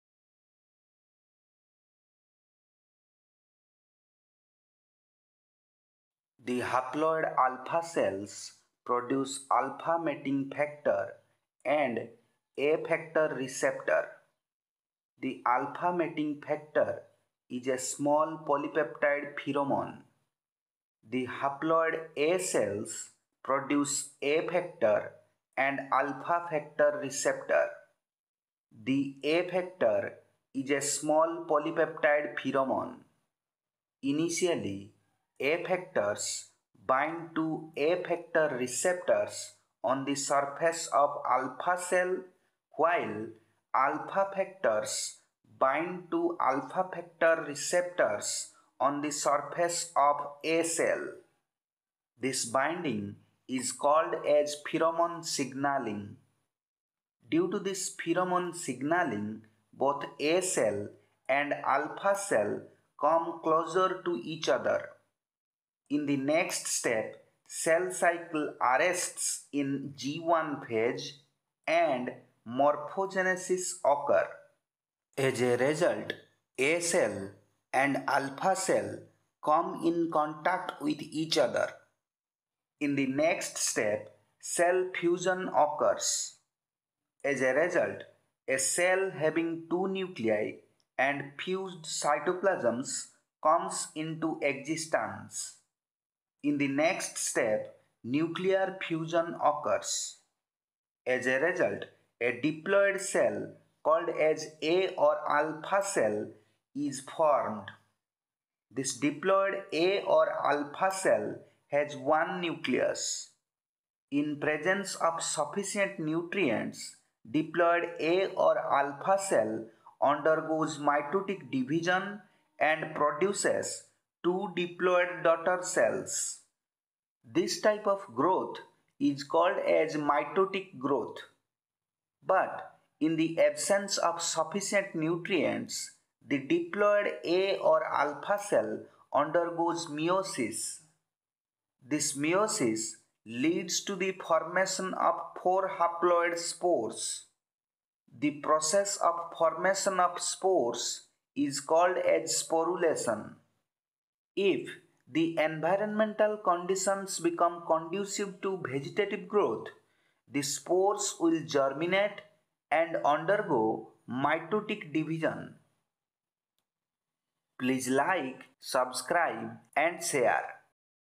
Pheromone induced mating of haploid yeast cells
In this video we have discussed about the pheromone induced mating of haploid yeast cells .